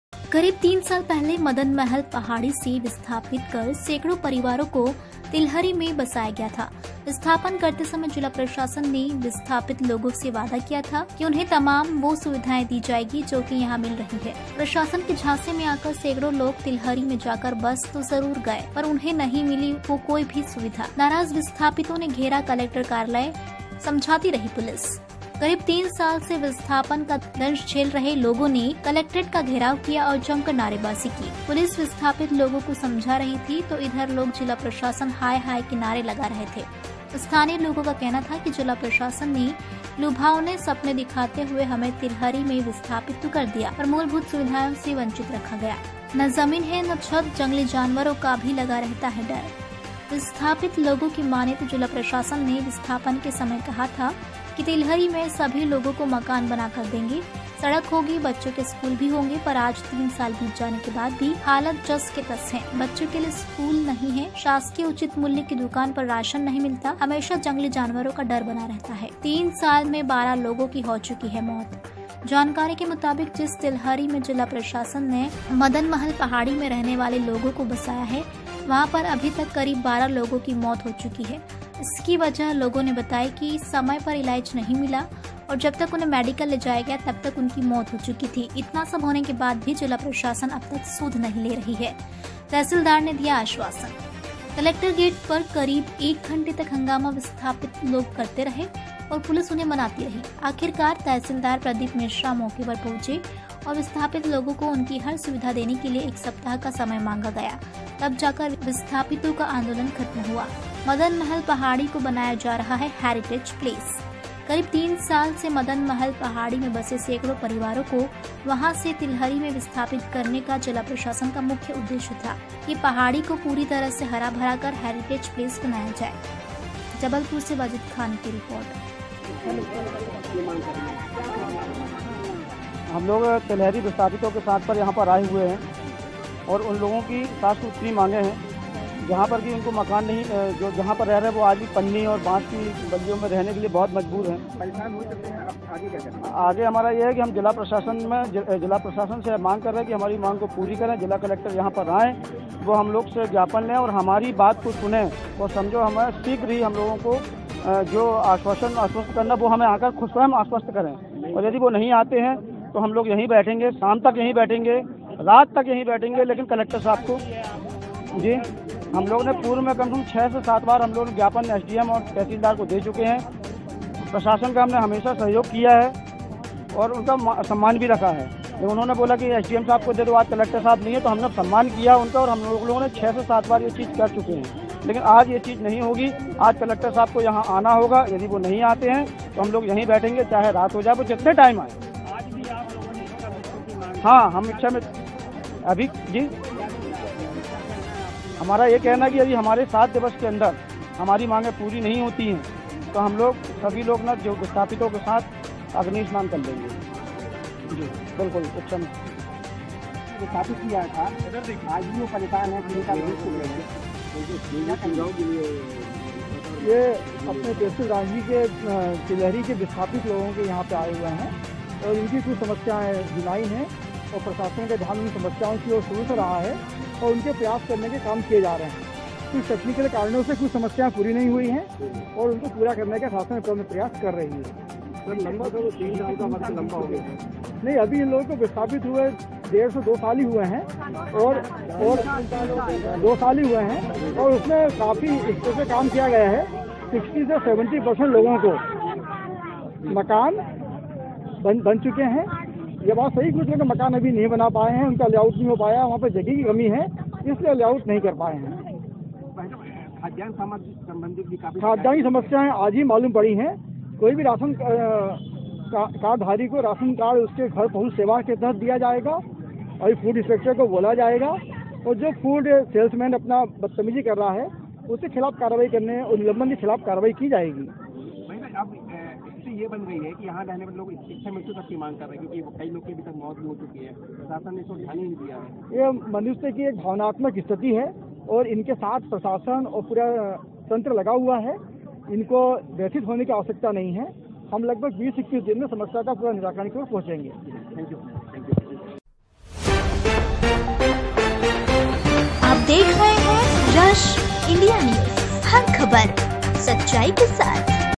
जबलपुर तिलहरी में विस्थापितयो ने किया हंगामा, कलेक्ट्रेड गेट पर कलेक्ट्रेटहाय-हाय के नारे
जबलपुर
तिलहरी में विस्थापितयो ने किया हंगामा,